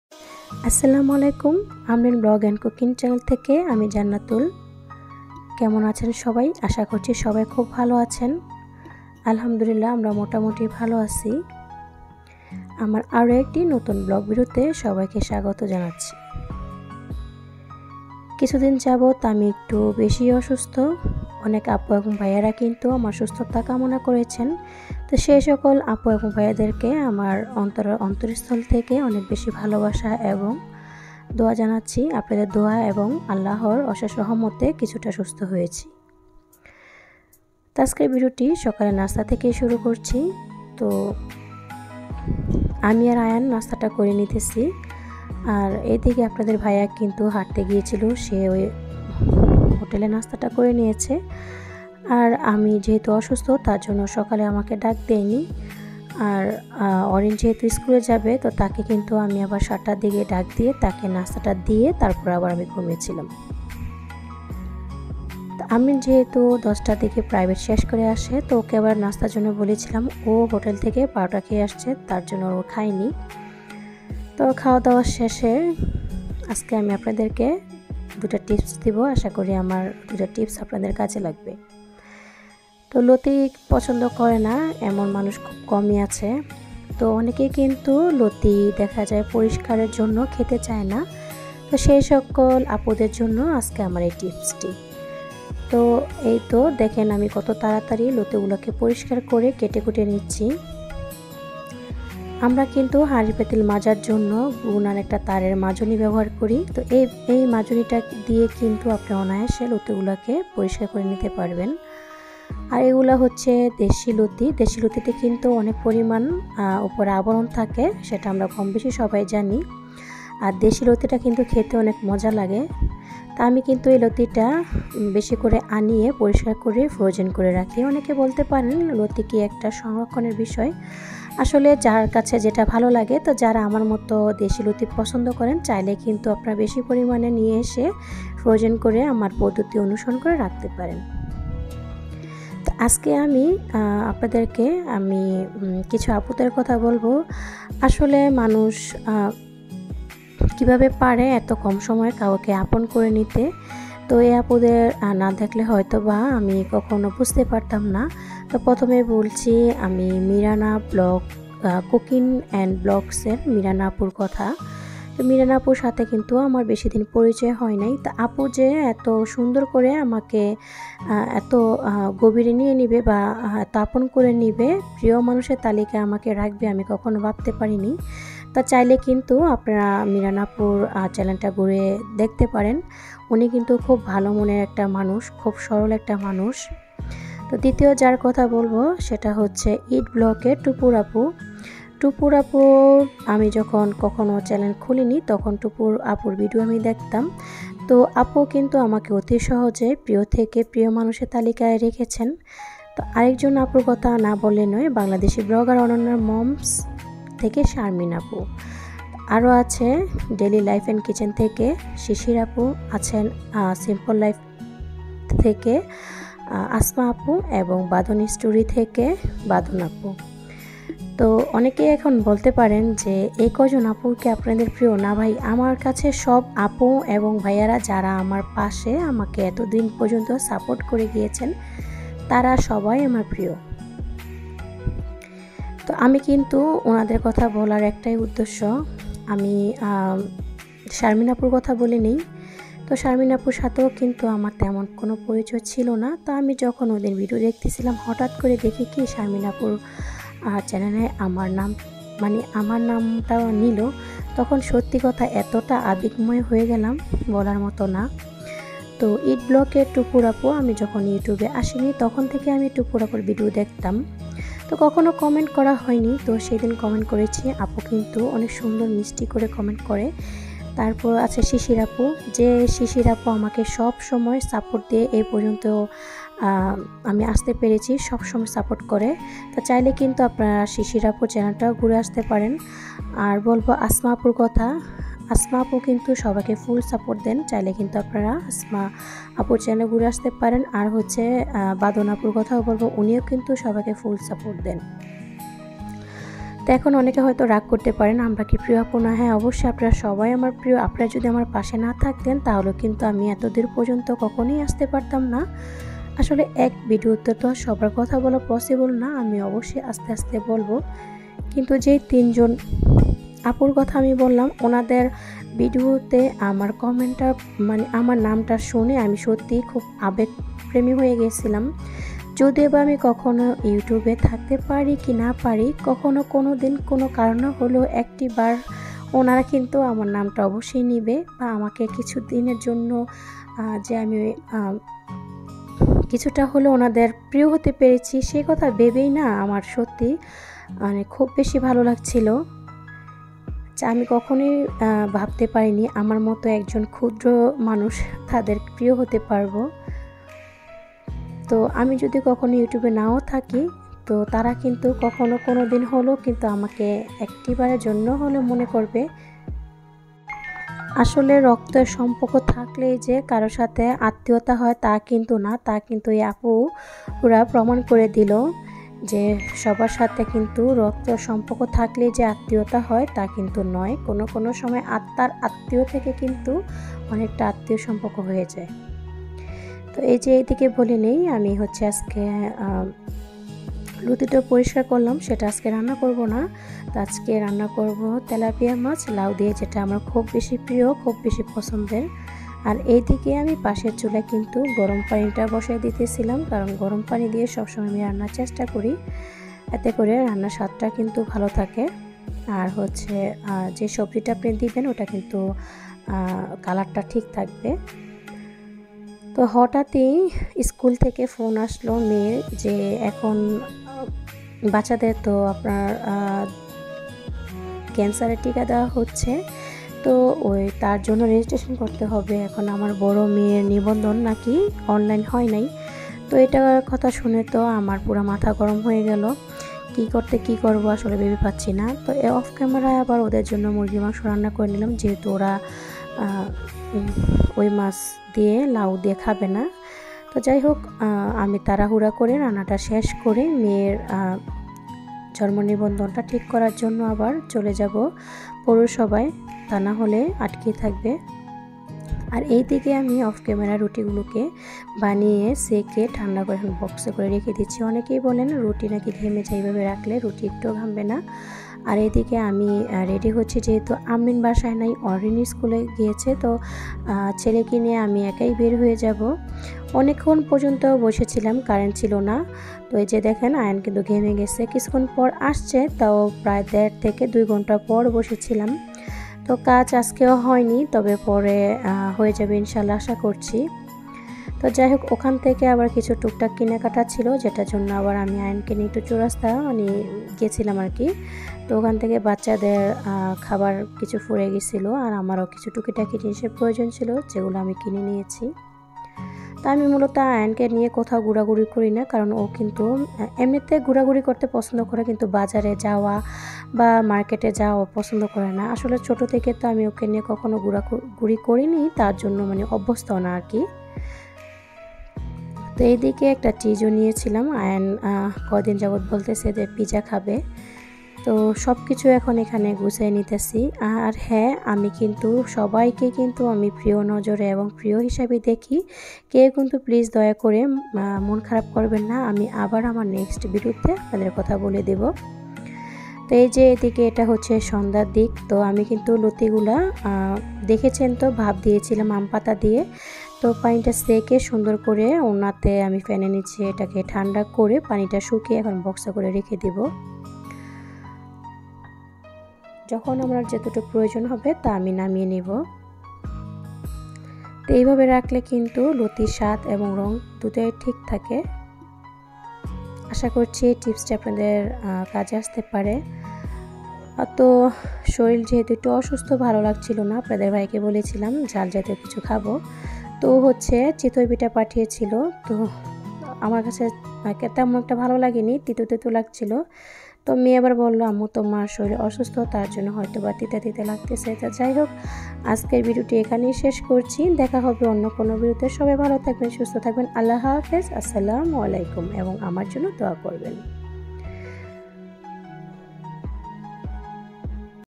রক্তের সম্পর্ক ছাড়াও কিন্তু আত্মার সম্পর্ক হয়।। লতি পরিষ্কার পদ্ধতি। লতি সংরক্ষণ পদ্ধতি।
রক্তের সম্পর্ক ছাড়াও কিন্তু আত্মা সম্পর্ক হয়। লতি পরিষ্কার করা।লতি সংরক্ষণ করা।